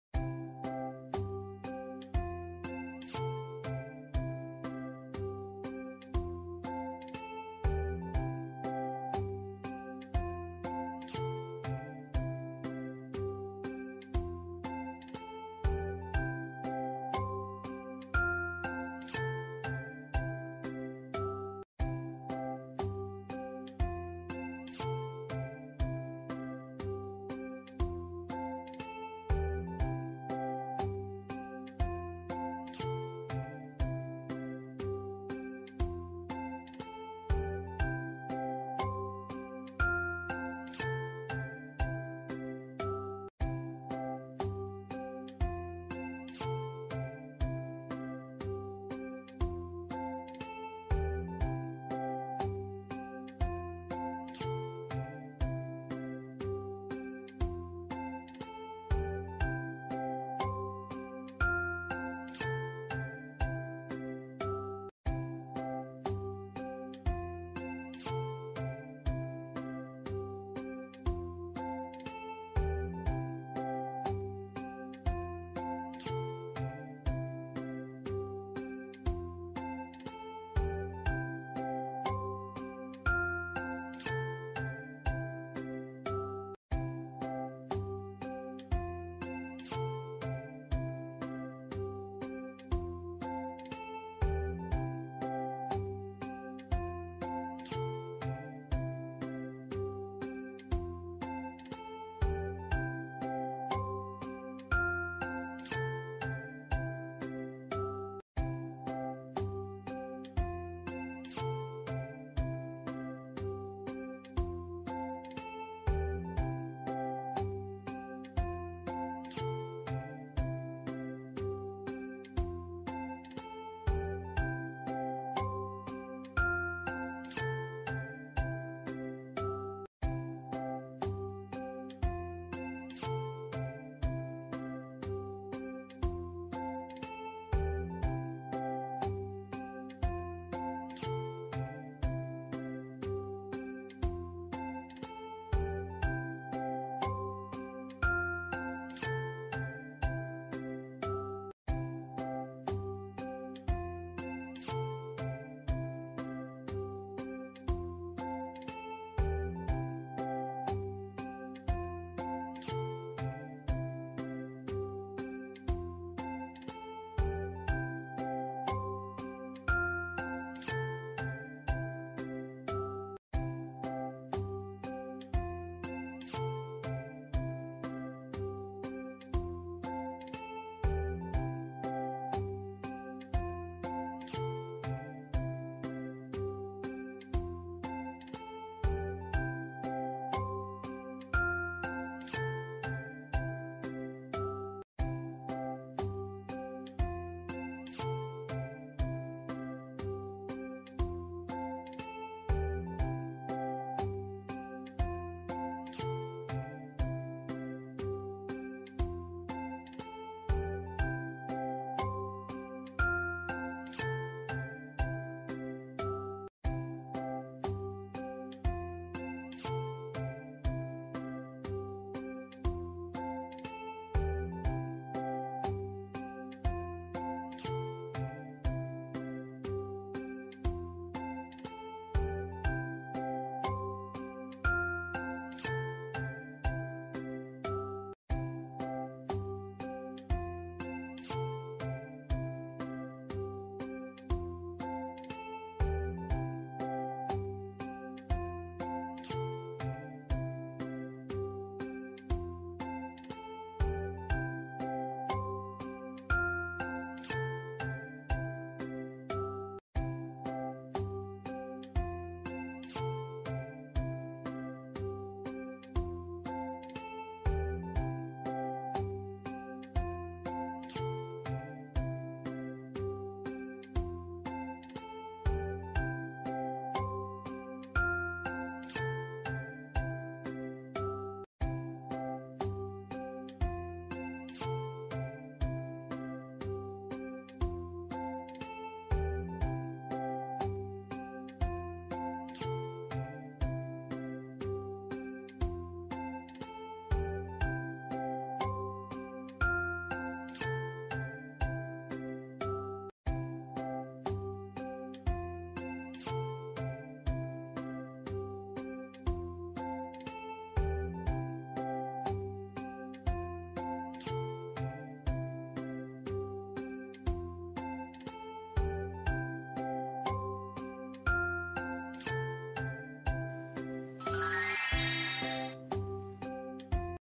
Full Hitam 🖤 Yuk Dekorasi Jurnal bersama Kuromi
dekorasi jurnal cantik menggunakan stiker lucu serba hitam dari kuromi, cocok bagi kamu yang suka warna gelap. Stay tune yaa!